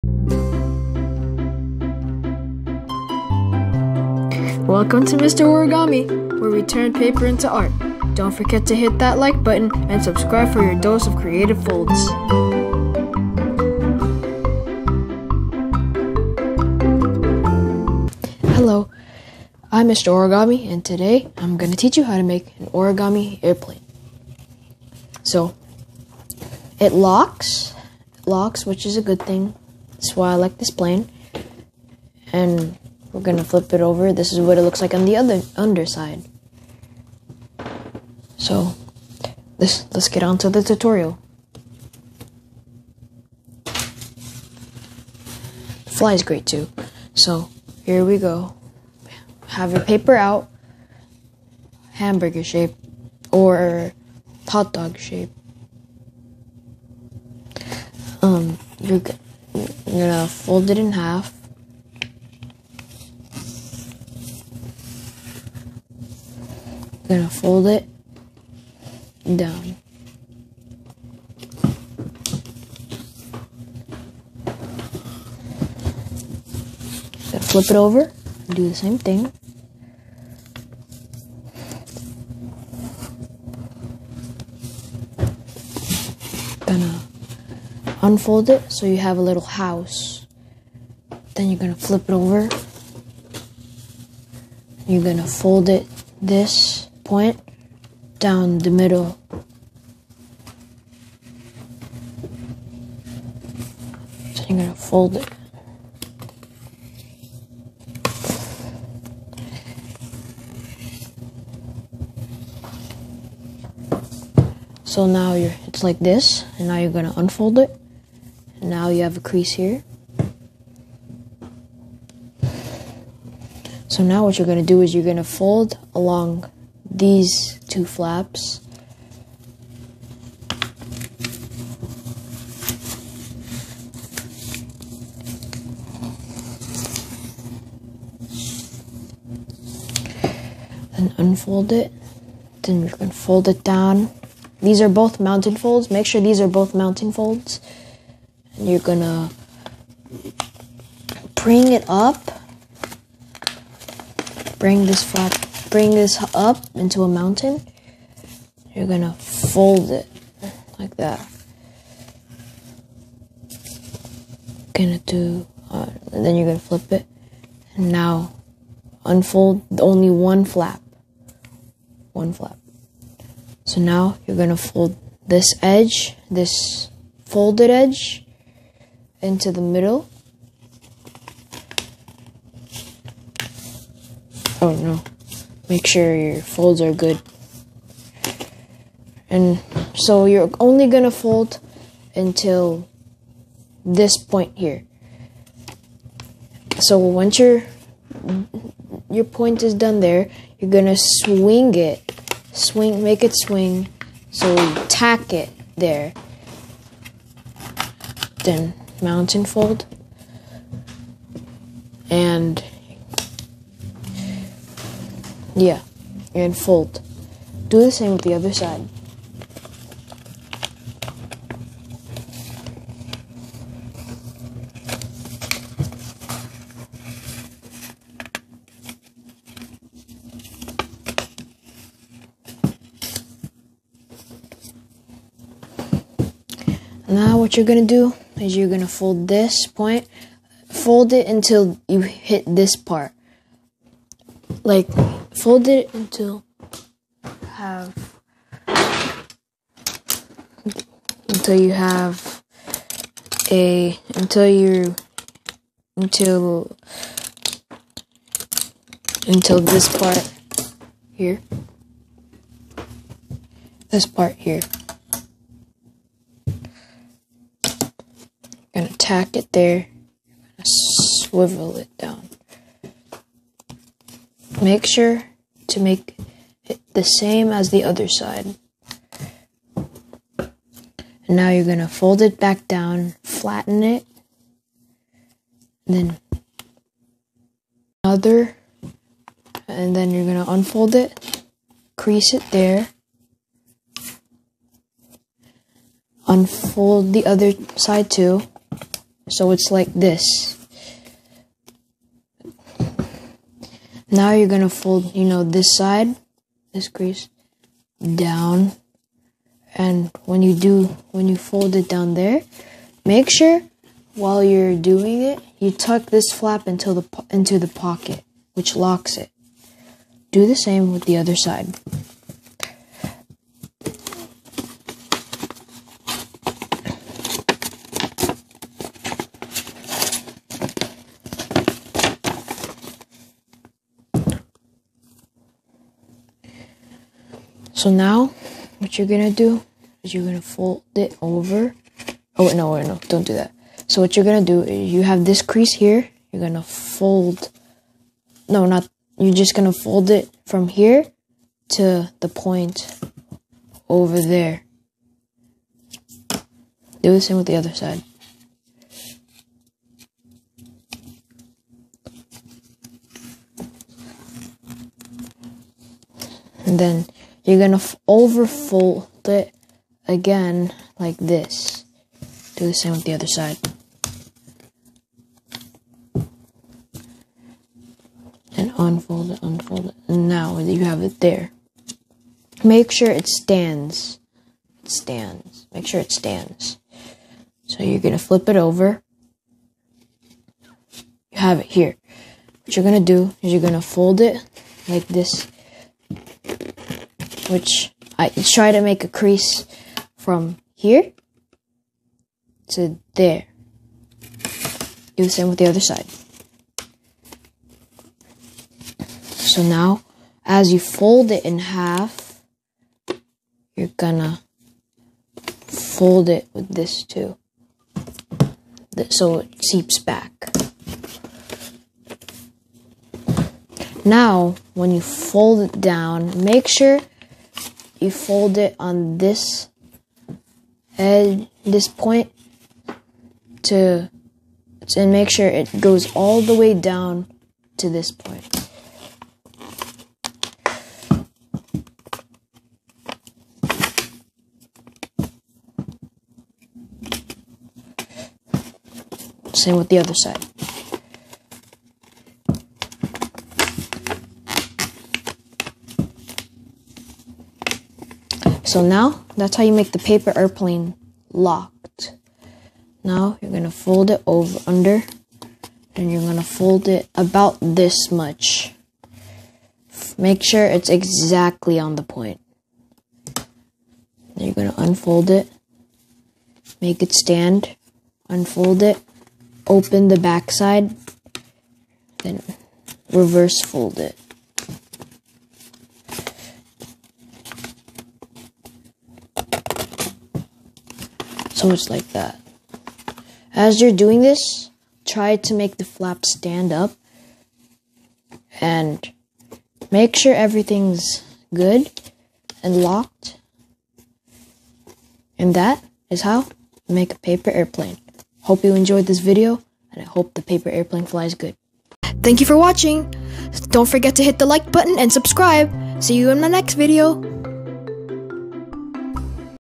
✈️ Origami - How To Make a Paper Airplane
Learn the art of crafting a paper airplane with this step-by-step tutorial!